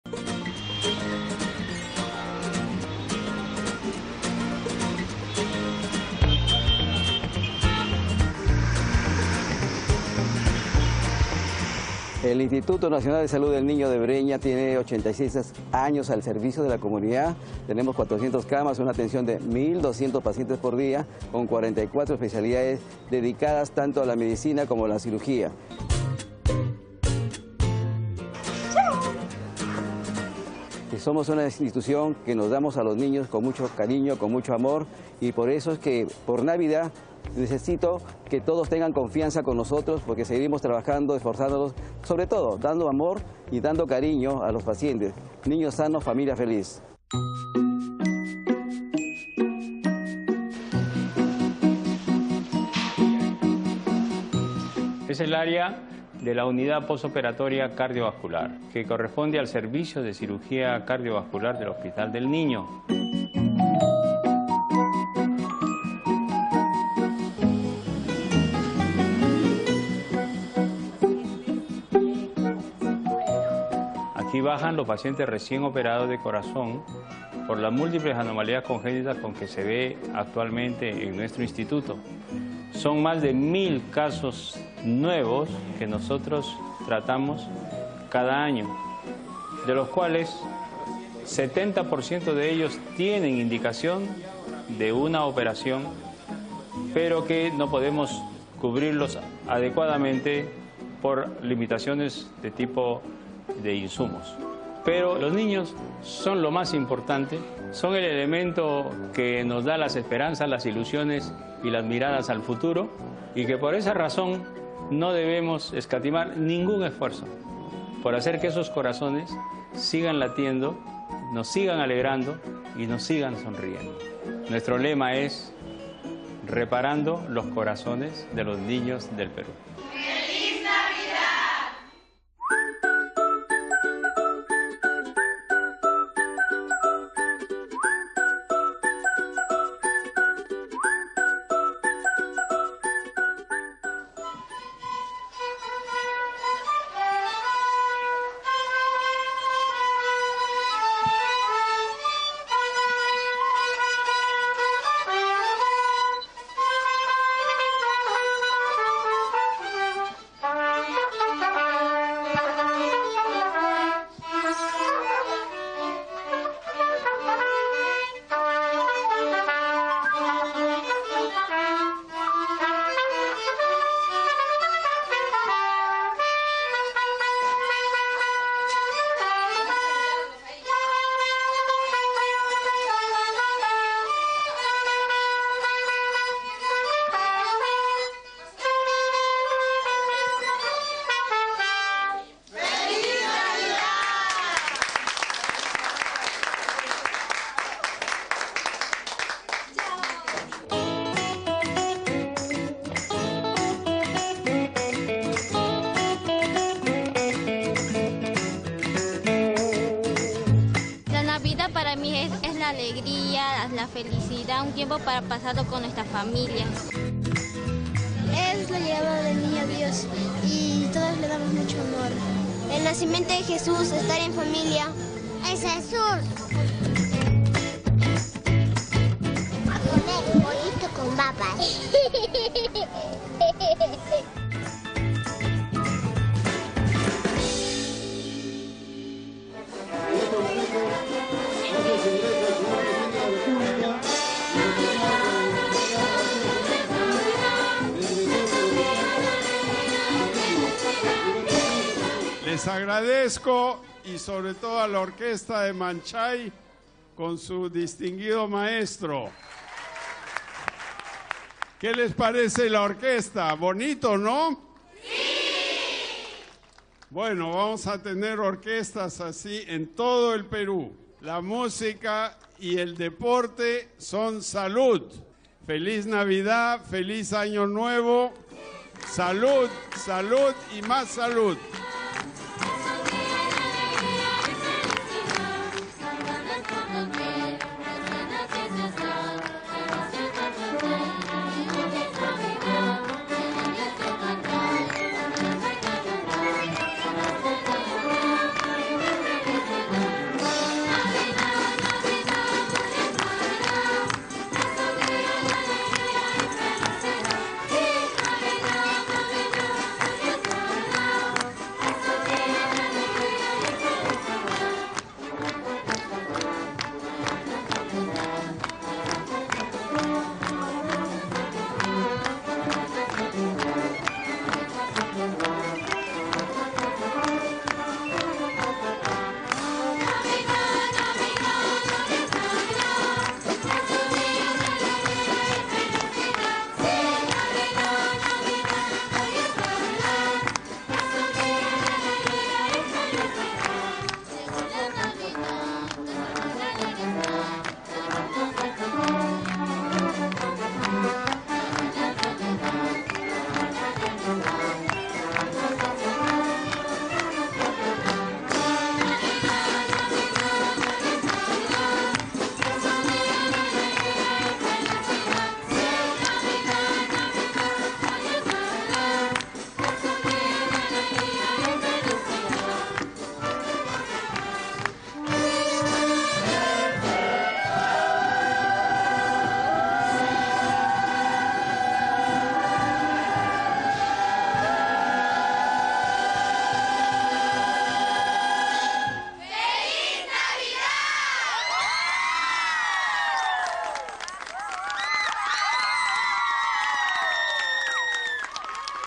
Kuczynski celebró Navidad anticipada con niños del INSN de Breña
El presidente Pedro Pablo Kuczynski llegó al Instituto Nacional de Salud del Niño para dar sus saludos por Navidad.